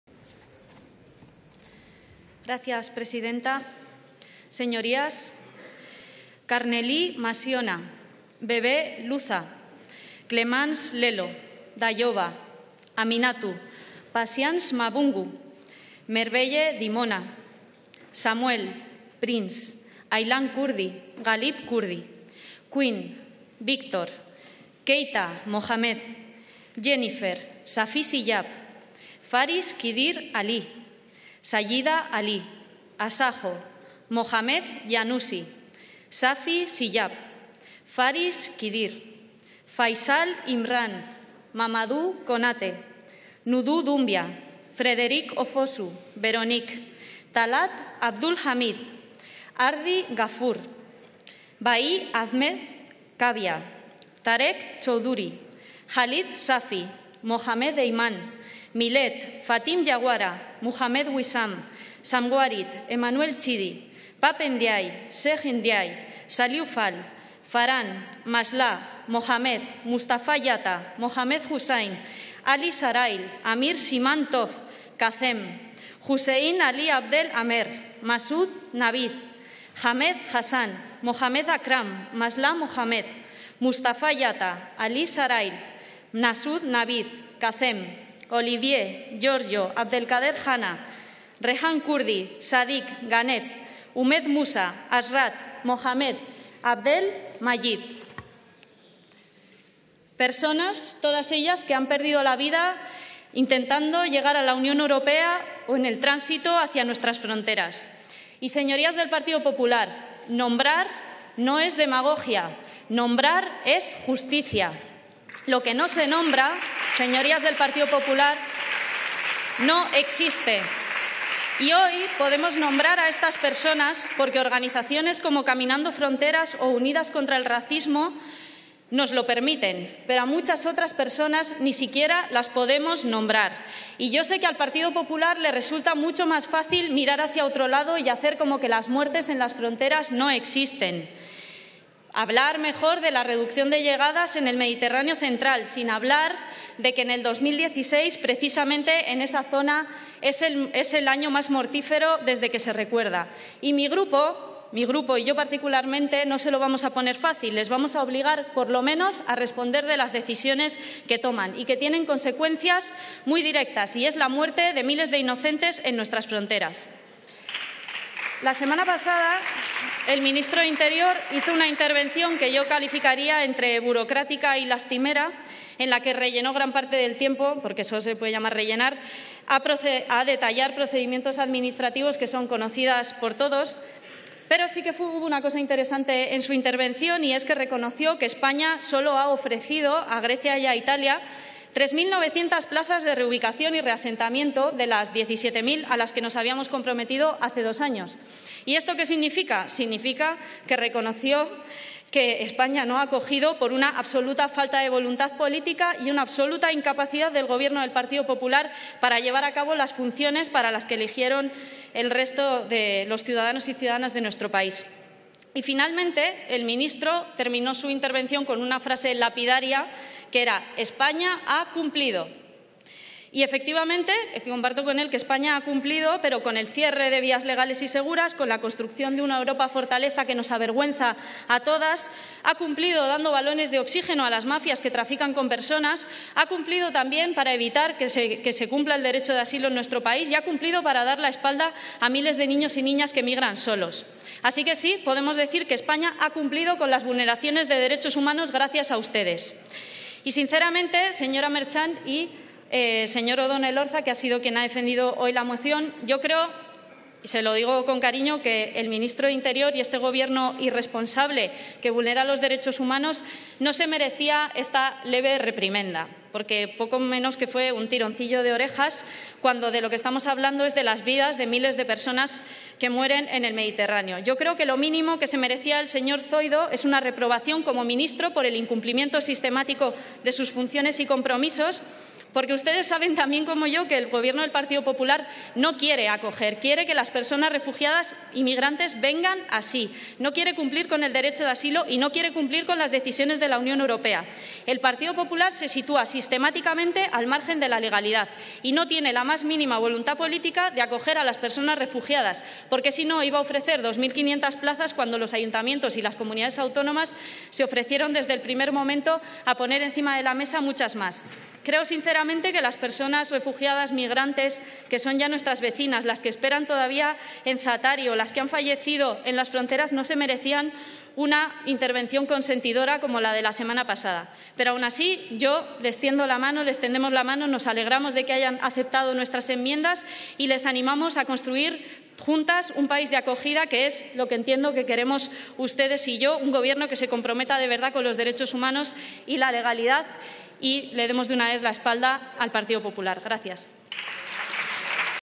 Ione Belarra: "Podemos decir que España ha cumplido con la vulneración de los Derechos Humanos"
En la Sesión Plenaria el 10 de octubre